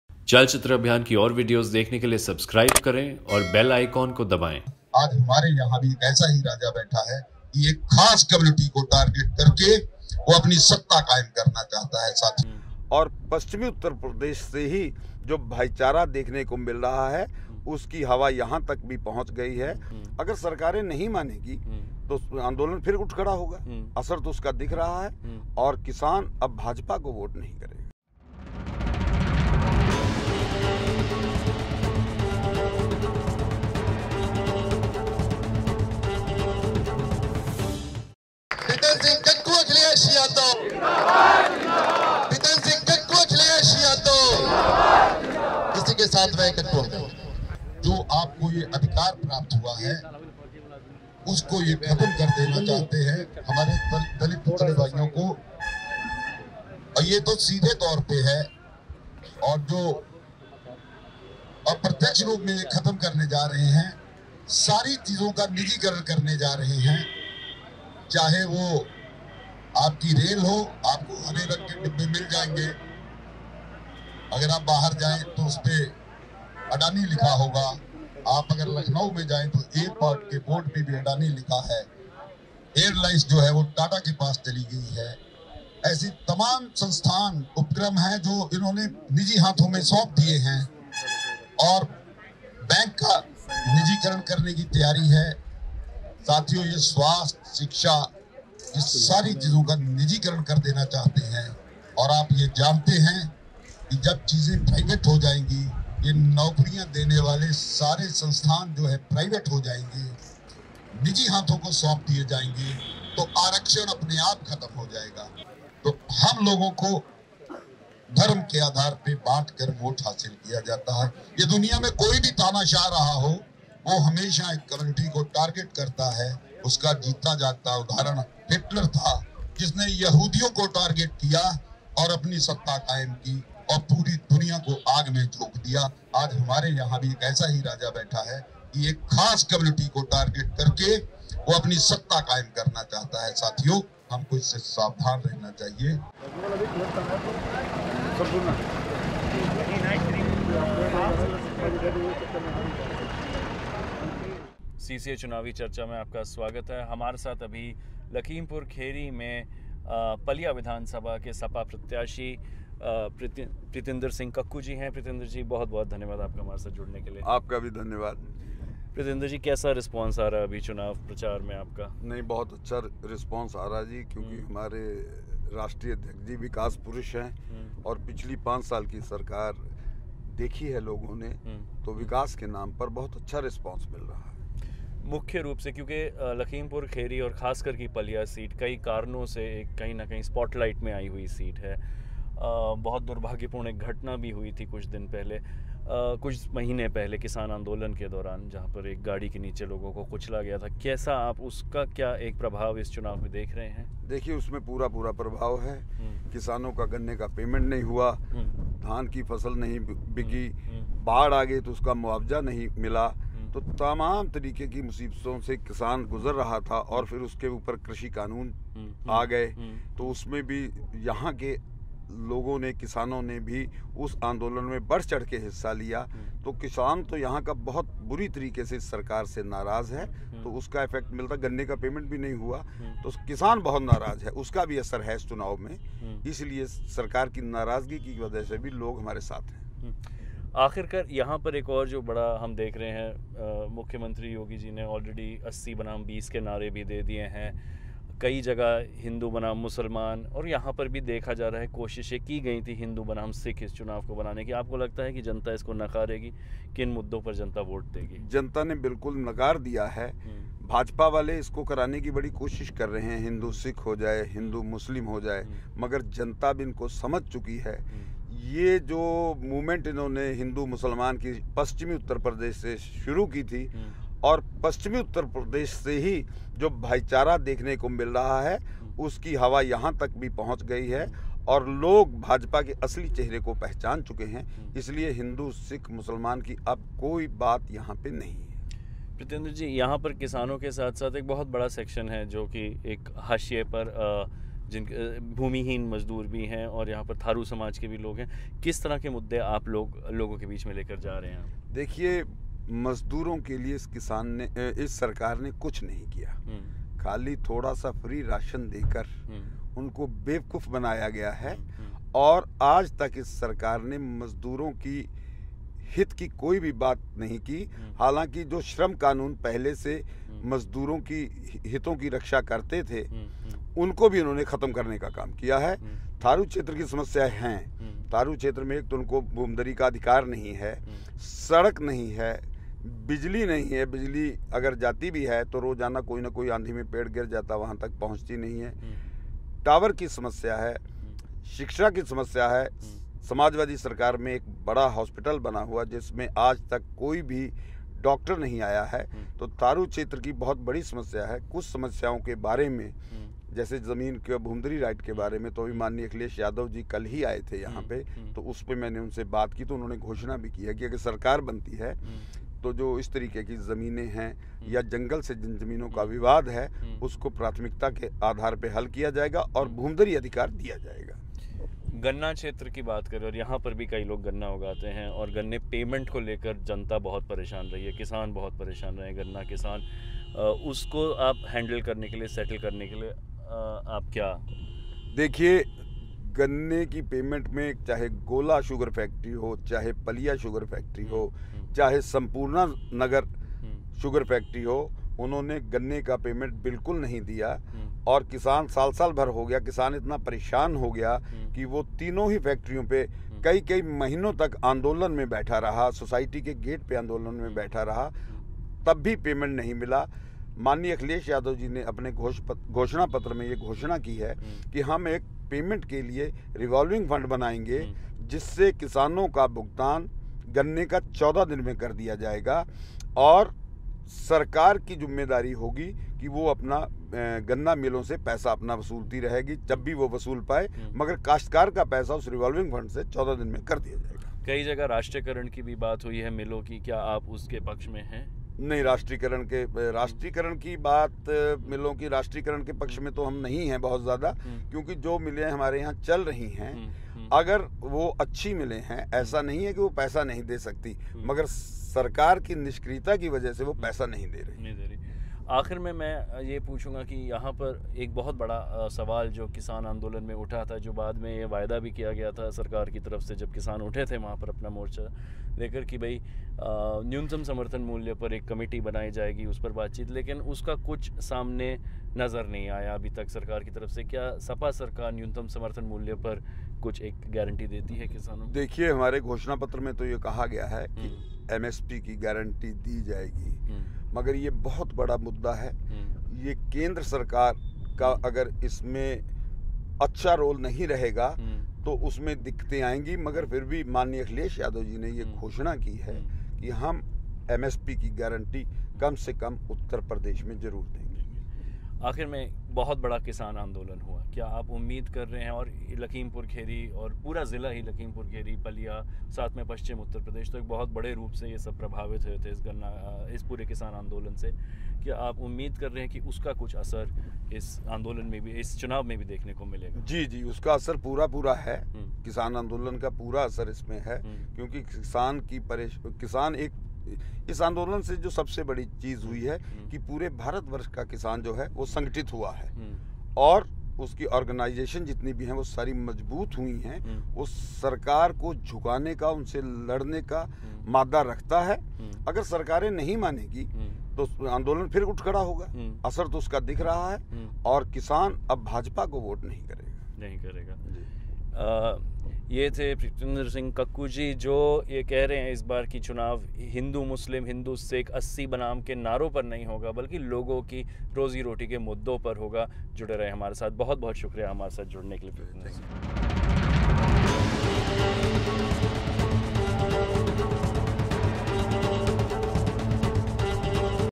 पलिया से सपा प्रत्याशी प्रीतिंदर सिंह कक्कू का इंटरव्यू | Interview: SP candidate Preetinder Singh
चलचित्र अभियान ने उत्तर प्रदेश के ज़िला लखमीपुर खीरी के विधानसभा सीट पलिया से सपा प्रत्याशी प्रीतिंदर सिंह कक्कू से बात की। प्रीतिंदर सिंह कक्कू का कहना हैं कि भाजपा सरकार सिर्फ़ नफ़रत की राजनीति करते हैं और किसान तो इस बार भाजपा को वोट नही देगा।
चलचित्र आभियान की रिर्पोट।
टीम- मोहम्मद सावेज चौहान, नकुल सिंह साहनी, विशाल स्टोनवॉल, अलनवाज आलम, शाकिब यामीन रंगरेज़

ChalChitra Abhiyaan spoke to Preetinder Singh Kakku, SP candidate from Palia Assembly seat in Lakhimpur Kheri district of Uttar Pradesh. He said that the BJP government does only hates politics and the farmers will not vote for BJP this time.
Crew -Mohd Savez Chauhan , Nakul Singh Sawhney , Vishal Stonewall, Alnawaz alam, Shakib Yameen Rangrezz